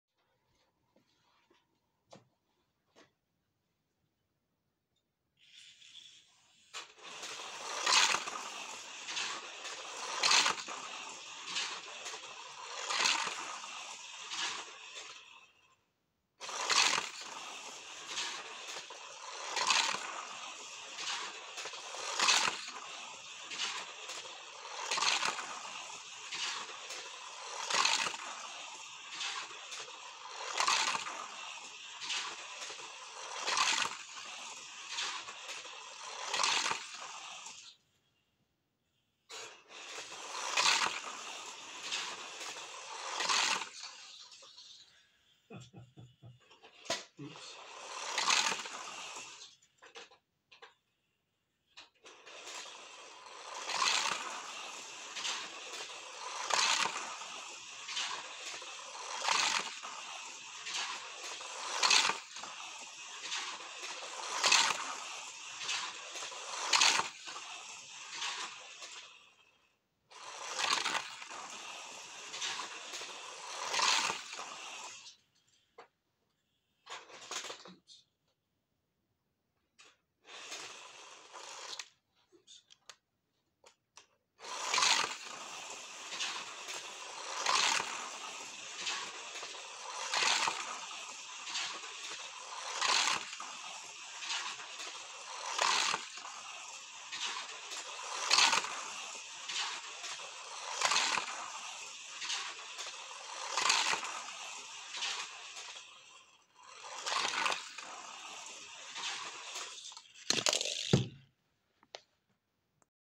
A 2021 Matchbox Scorpion Ford Mustang in full flight.
A RayCad design Matchbox Scorpion Ford Mustang for the 21st century!